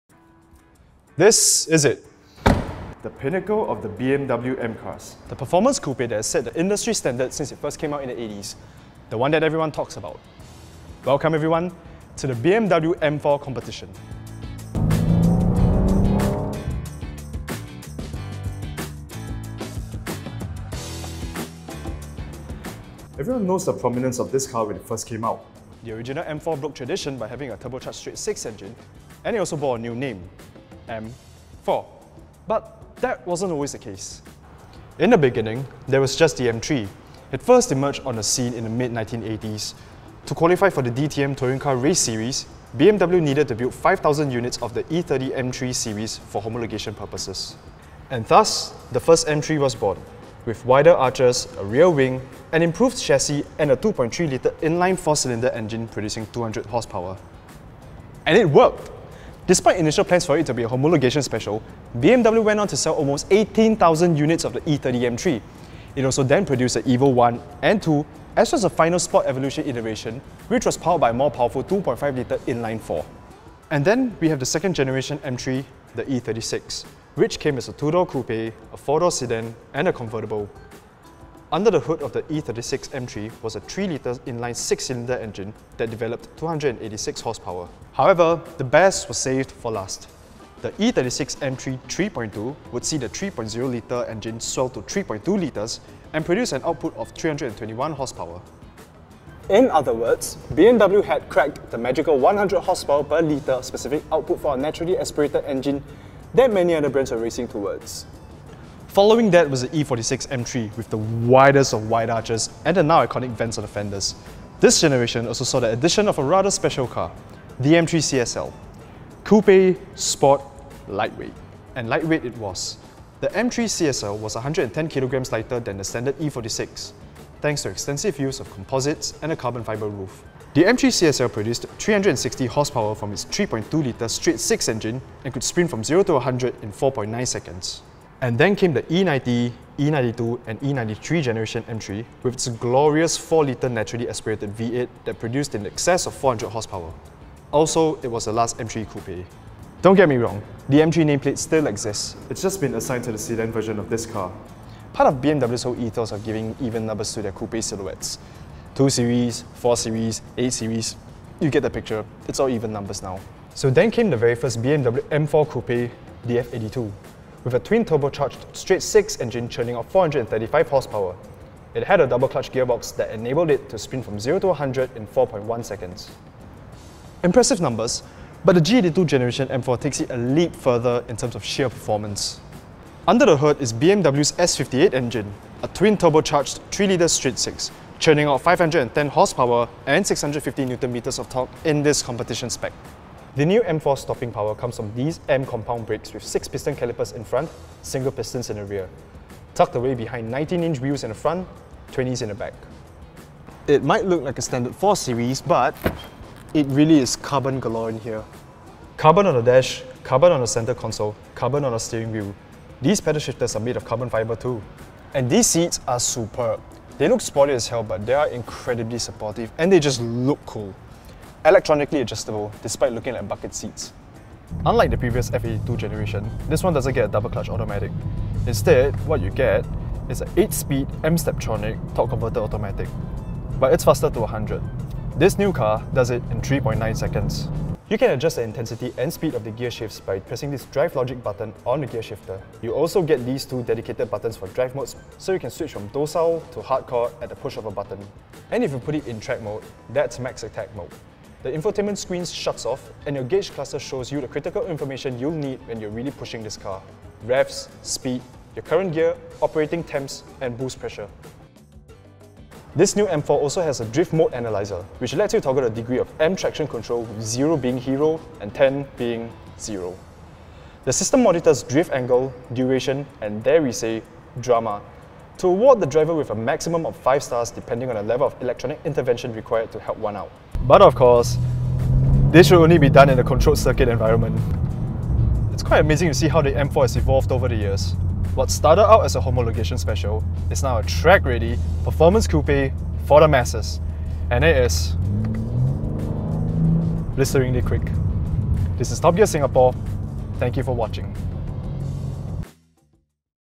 BMW M4 Competition: An M-Four The History Books | TopGear Singapore
The iconic BMW M4’s lineage stretches all the way back to the E30 M3 of the 1980s.

00:00 Opening
00:22 History of the BMW M4
00:34 The original E30 M3
01:13 The E36 M3
01:48 The E46 M3
02:18 The last M3 Coupe, the E92
02:43 The first M4, the F82-Generation
02:56 Performance of the G82 M4
03:23 Interior of the G82 M4
03:44 G82 M4 Driving experience
04:45 Final thoughts and closing